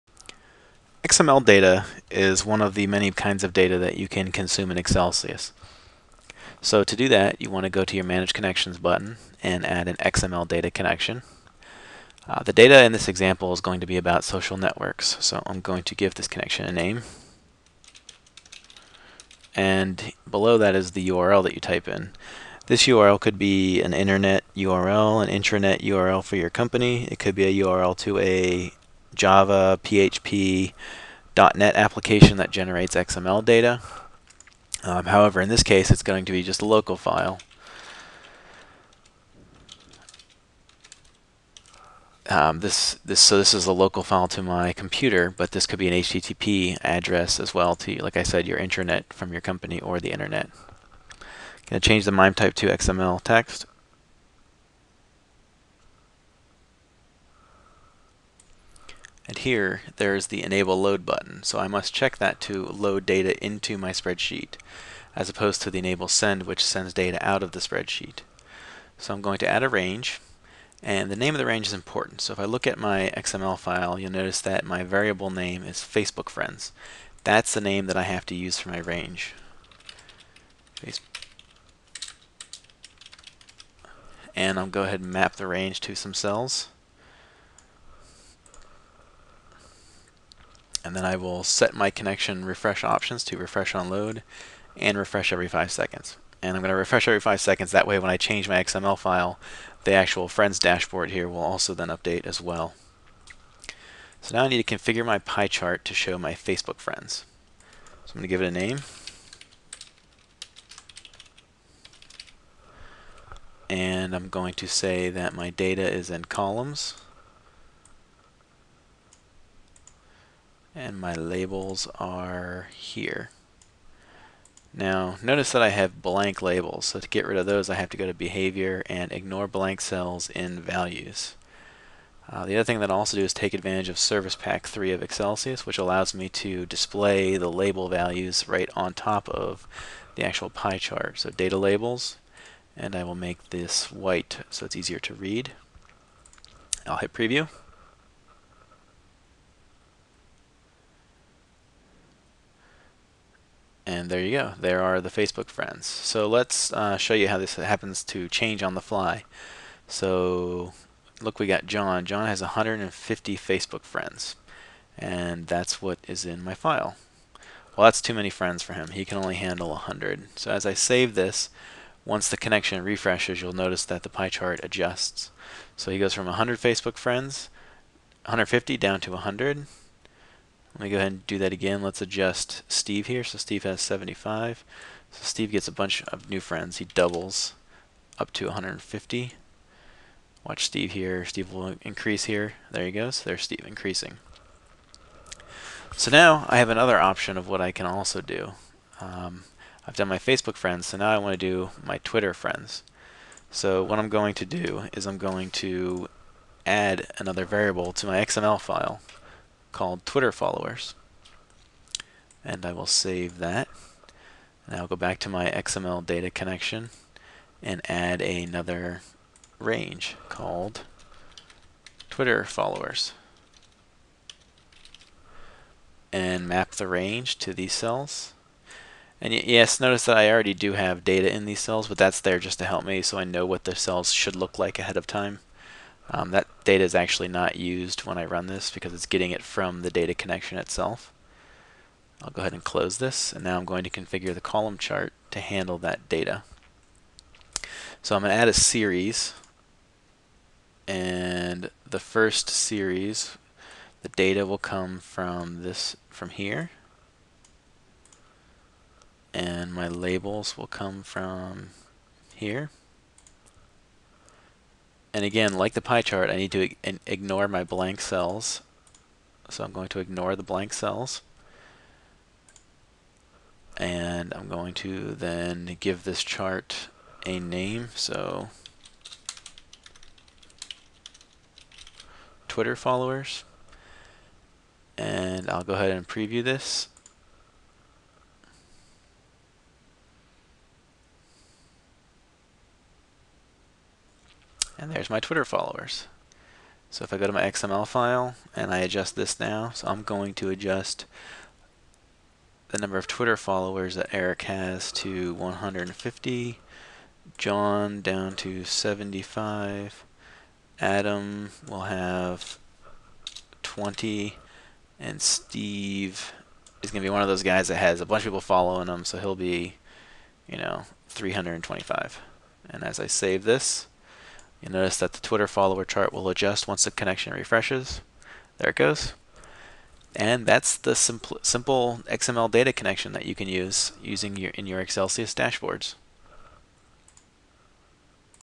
Consuming XML Data in Xcelsius Using XML Data Connection
One of the many connection types in Xcelsius is the XML Data connection. You may think XML data is boring but there are a lot of benefits to using XML data. XML data can be made available by manually typing an XML file or automatically generated by an application or database. The video below shows an example of how to consume simple XML data using Xcelsius.  When writing the out manually or by developing a program, remember that the "variable" element requires a name property that matches the range name in Xcelsius.